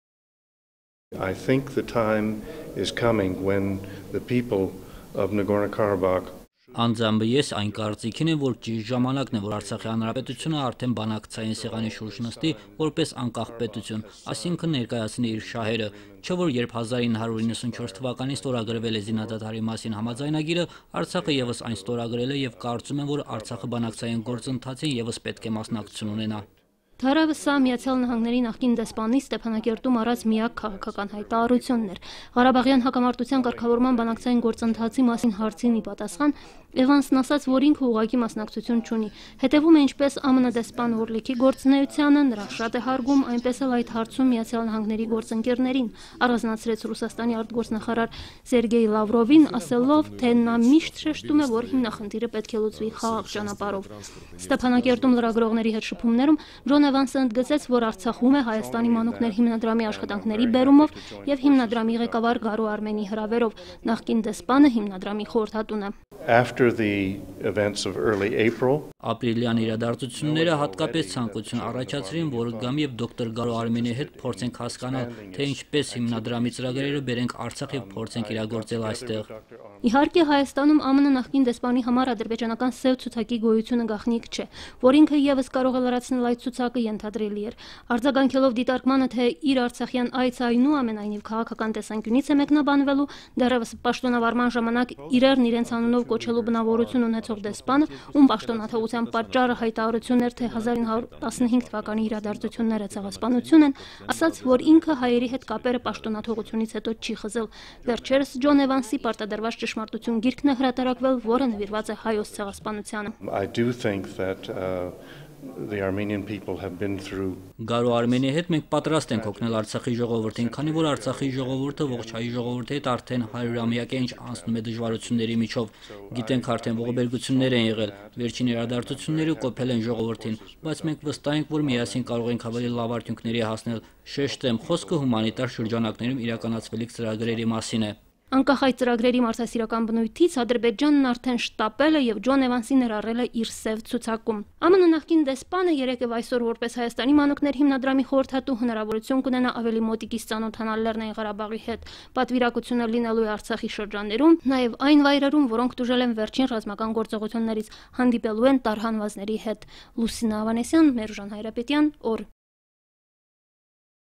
Ճիշտ ժամանակն է, որ Արցախի Հանրապետությունը բանակցային սեղանի շուրջ ներկայացնի իր շահերը
″Ճիշտ  ժամանակն է, որ Արցախի Հանրապետությունը բանակցային սեղանի շուրջ  որպես անկախ  պետություն ներկայացնի իր շահերը″,- այս կարծիքին է Հայաստանի Հանրապետությունում ԱՄՆ նախկին դեսպան Ջոն Էվանսը, ով Լեռնային Ղարաբաղի Հանրապետություն է այցելել  ″Հայաստանի մանուկներ″  հիմնադրամի պատվիակության կազմում:

″Самое подходящее время, чтоб Республика Арцах села за стол переговоров в качестве независимого государства и представила свои интересы″,- такого мнения придерживается бывший посол США в Армении Джон Эванс, посетивший Нагорно-Карабахскую Республику в составе делегации Фонда ″Дети Армении″.